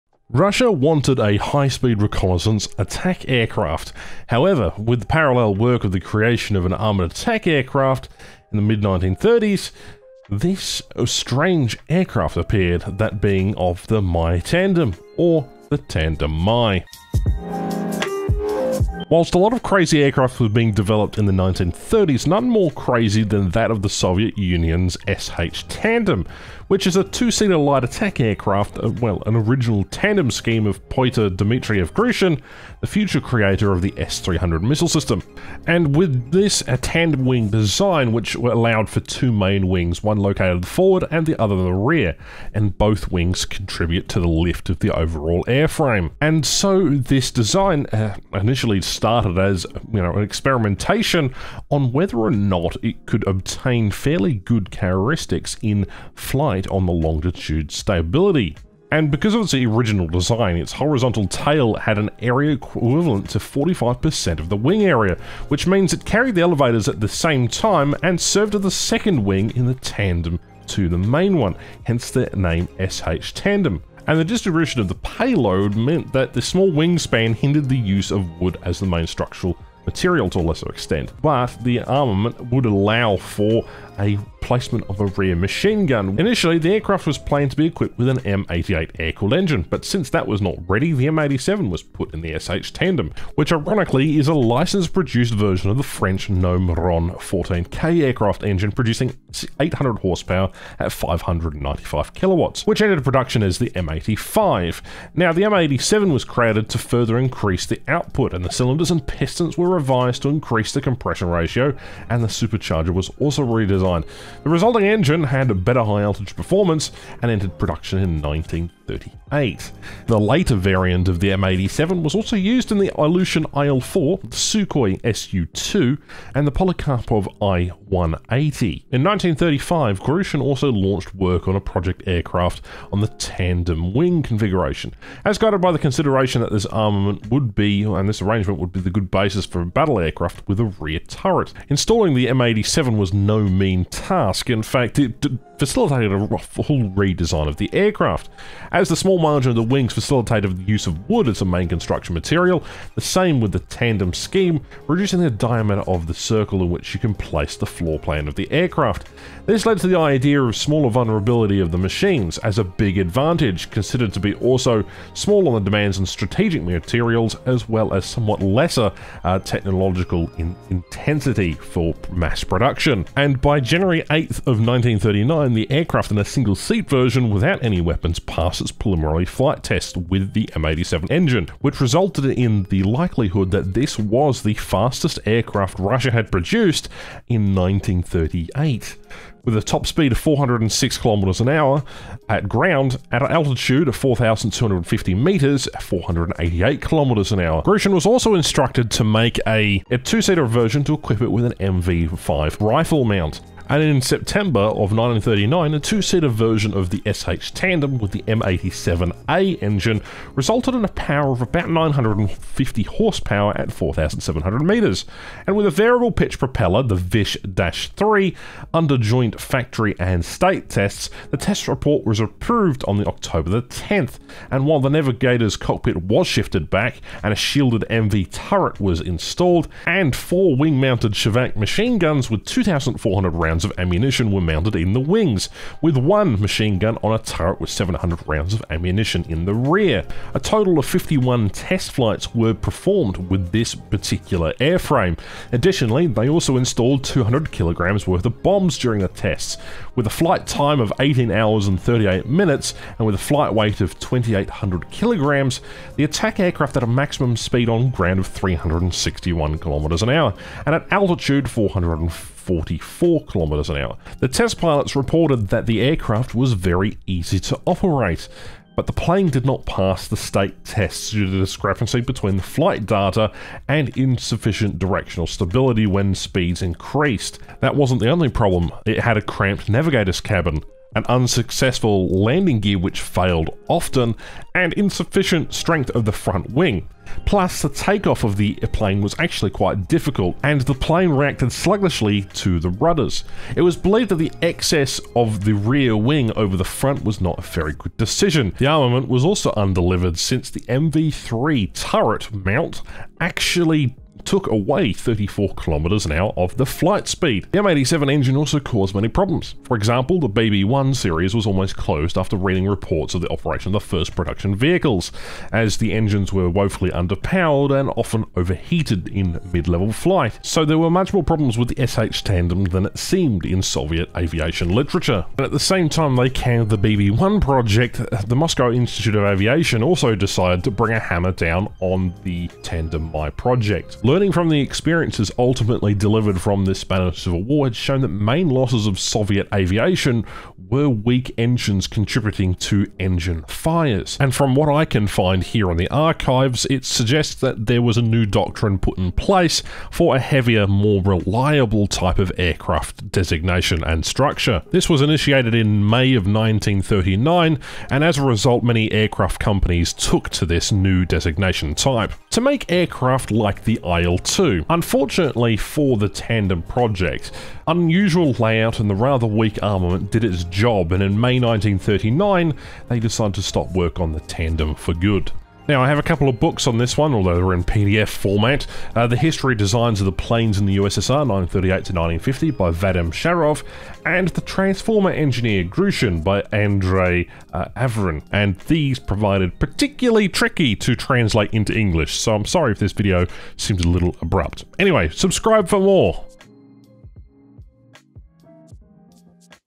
Russia's Tandem Wing Attack Aircraft
TheTandem MAI was an impressive design. This Russian aircraft was the first to utilise Tandem Wing configurations unfortunately it would be plagued by delays and ultimately the program would be killed by the IL-2 combined with other programs . Still one prototype was built and testing highlighted that the wings provided exelent lift for heavy payloads. However the aircraft never took off.

Sources:
– 'Ground Attack' by Christopher Chant. 'The Mechanics of War' series.
Almark Publishing Co Ltd London, 1976
– 'Notes of an aircraft designer' by Alexandre Yakovlev
– History of aircraft construction in the USSR", V.B.Shavrov